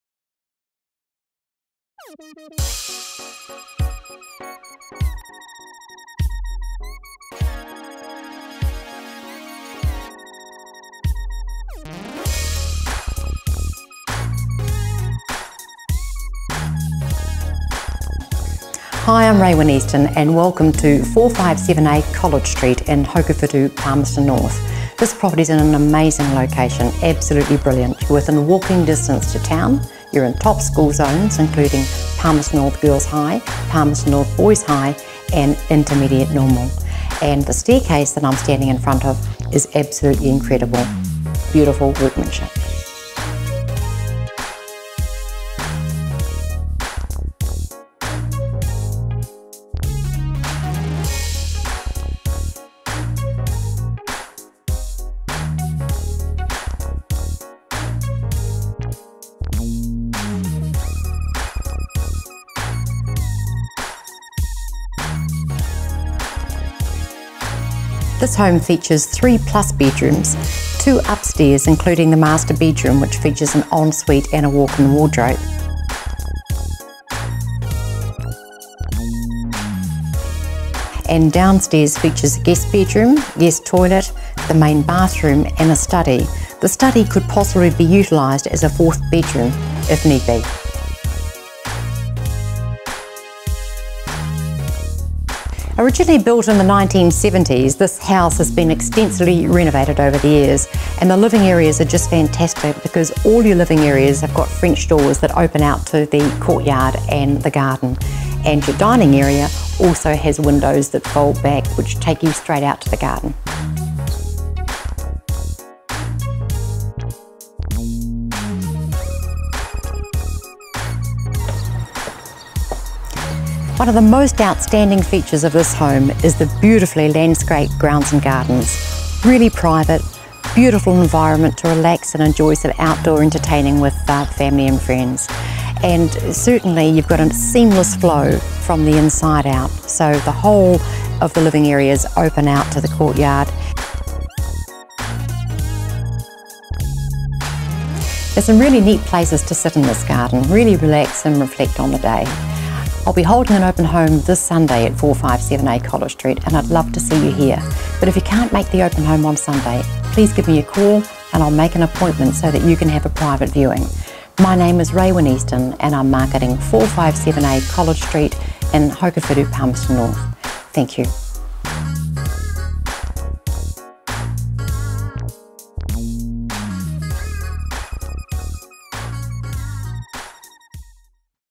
457a College St, Palmerston North
Walking distance to the city, a private, tranquil and immaculate landscaped property. Just needs to be seen to be fully appreciated what is on offer here.